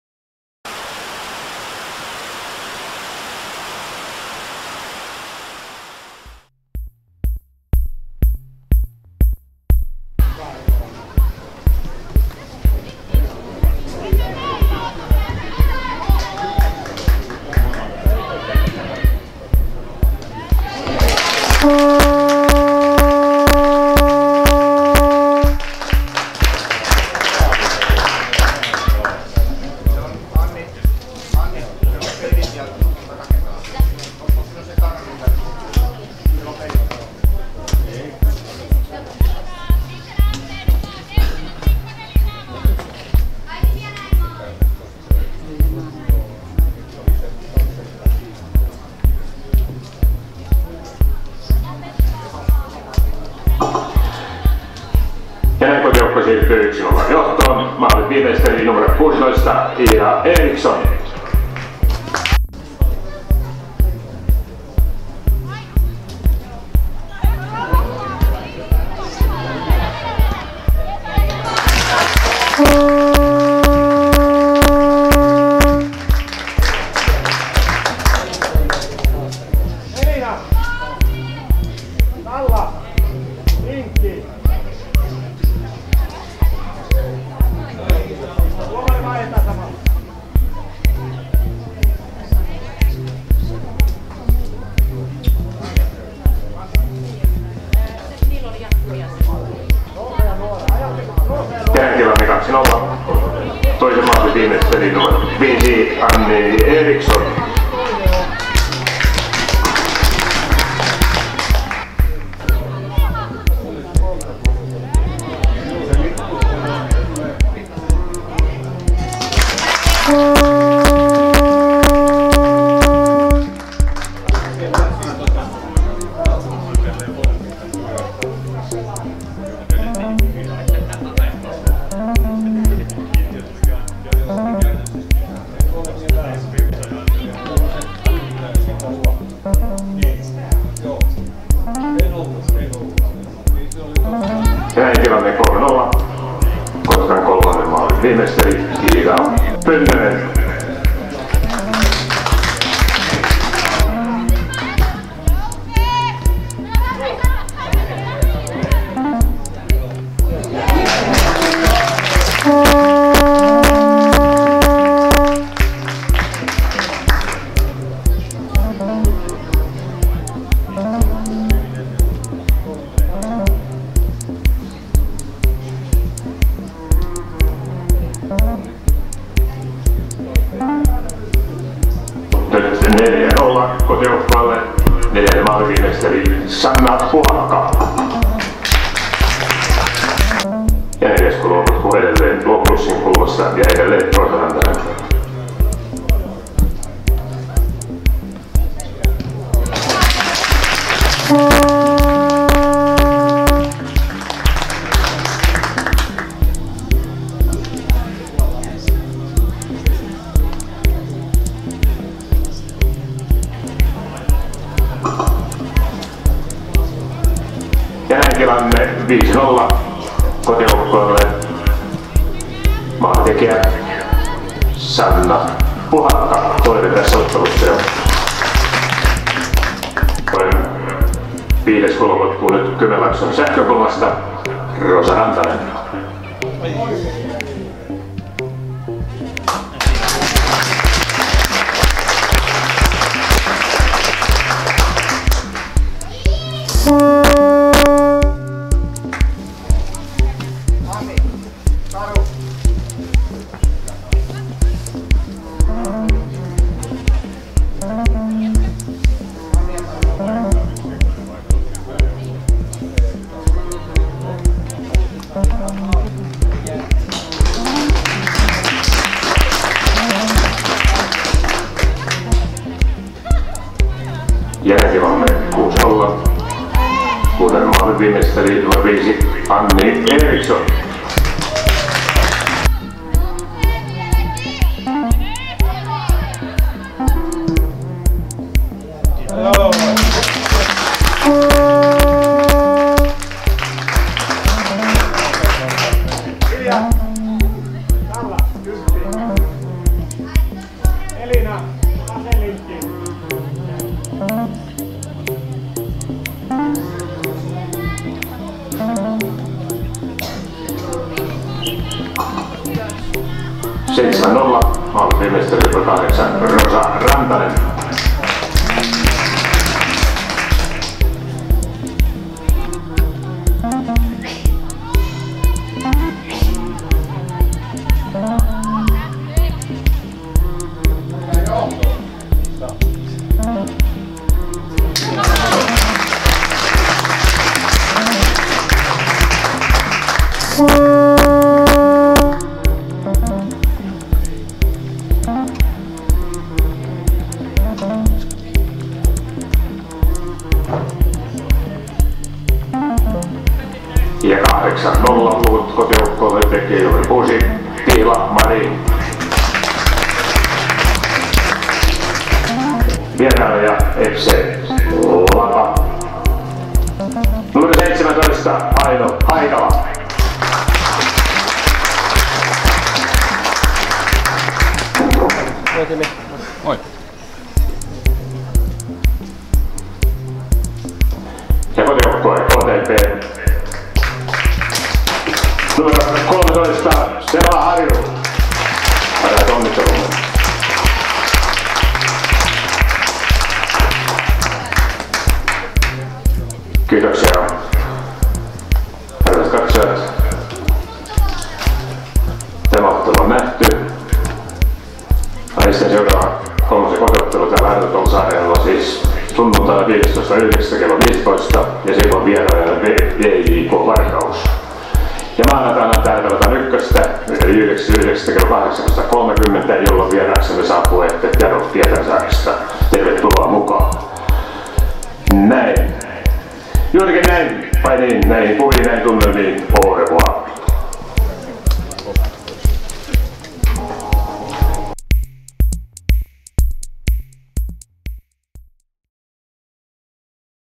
Kooste: 4.9.19 | FC KTP - FC LAPA | Naiset Kolmonen | Arto Tolsa Areena
Pahoittelut, teknisistäsyistä tässä koosteessa ei nähdä maalikameroista kuvaa.

Mielellämme saisimme palautetta katsojilta, mikä on hyvää ja missä parantamisenvaraa.